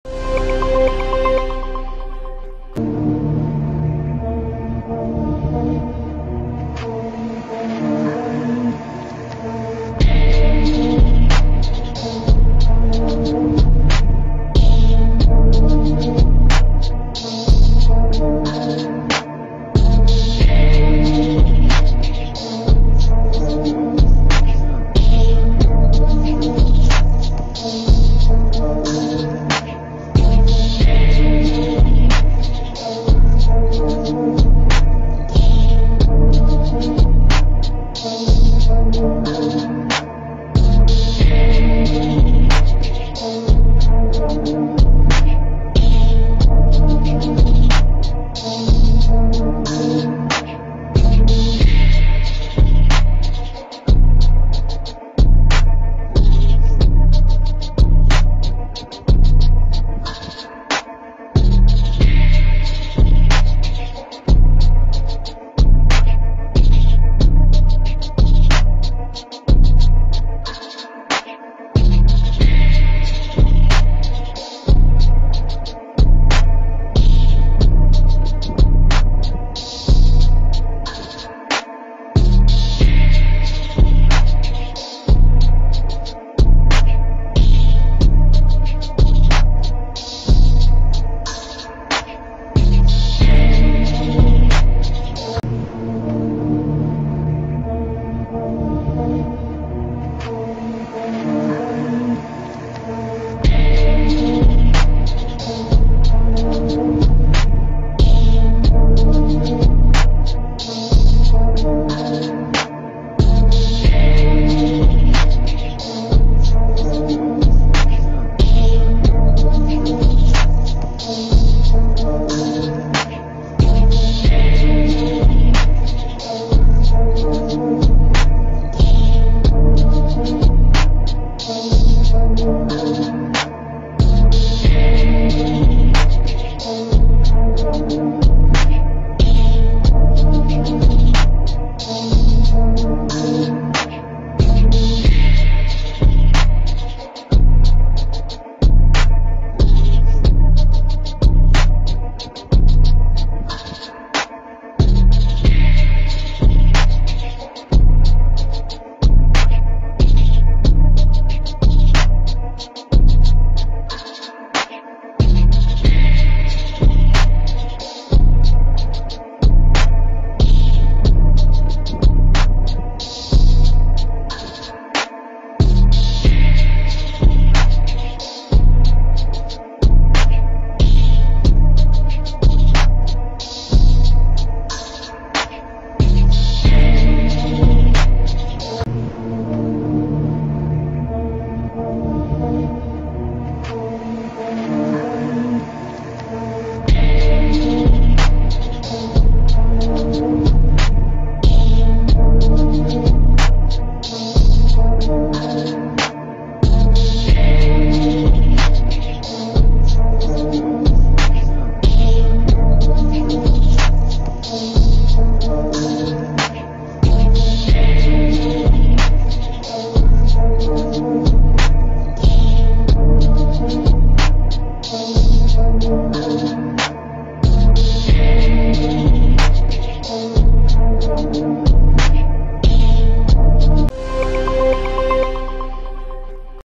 Russians who attacked in Donetsk with the tanks called "Turtle" were ambushed this time
Four tanks of the occupying Russian army called "turtles" together with infantry attacked the city of Krasnogovka, Donetsk region. After dropping the infantry, the tanks trying to back were hit by drone and artillery fire of the Ukrainian army. As a result, two tanks were destroyed in the city and one on the outskirts of the city. One of the hit tanks was removed from the area with the help of another tank.